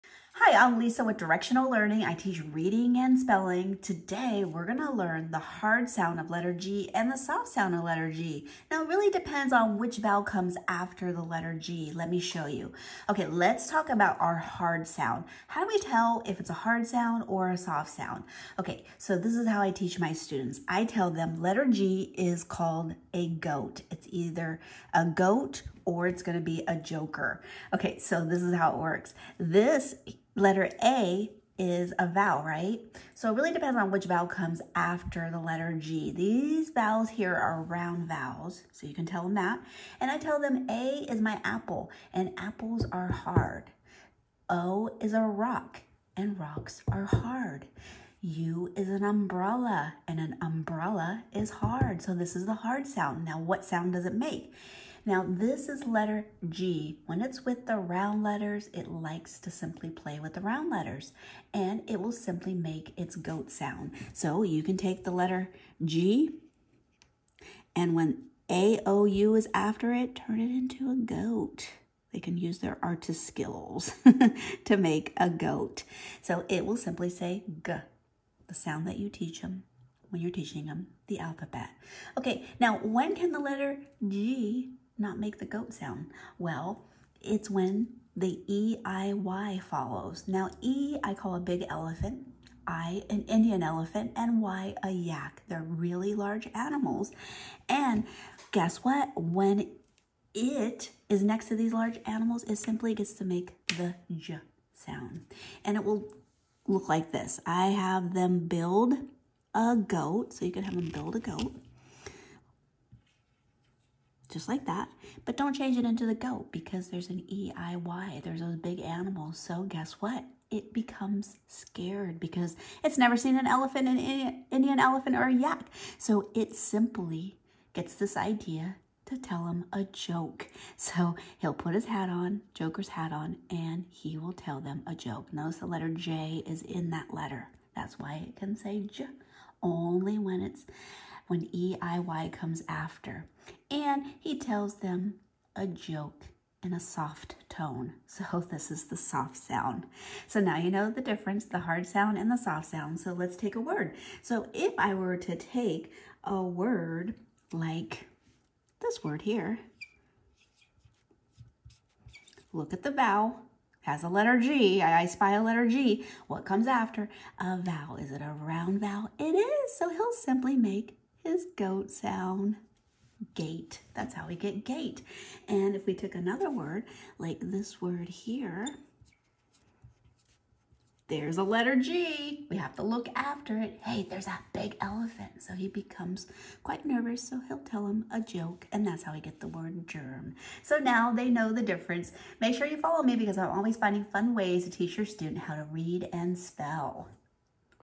July 6, 2024 : Mastering Phonics: Hard G vs. Soft G | Orton-Gillingham Reading Tips
Description:
In this educational video, we explore into mastering phonics by learning the difference between Hard G and Soft G sounds. Using a multi-sensory approach inspired by Orton-Gillingham methods, we provide effective teaching strategies and tips to help learners of all ages grasp these fundamental concepts. Whether you're a parent, teacher, or learner yourself, understanding phonics through this method can greatly enhance reading skills. Join us to unlock the secrets of phonics with clarity and confidence!